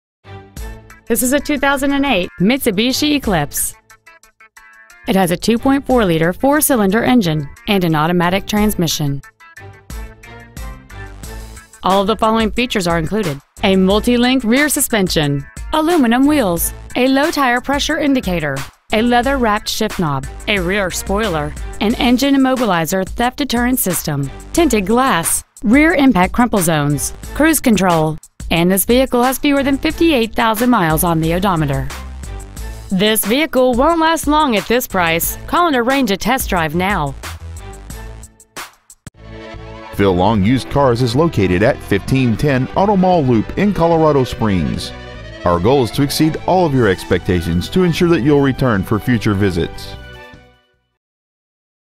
2008 Mitsubishi Eclipse Colorado Springs CO
Year : 2008
 Make : Mitsubishi
 Model : Eclipse
 Engine : 2.4L SOHC MPFI 16-valve MIVEC I4 engine
 Trans . : Automatic
 Exterior : Red
 Miles : 57,054

 Preowned 2008 Mitsubishi Eclipse Colorado Springs CO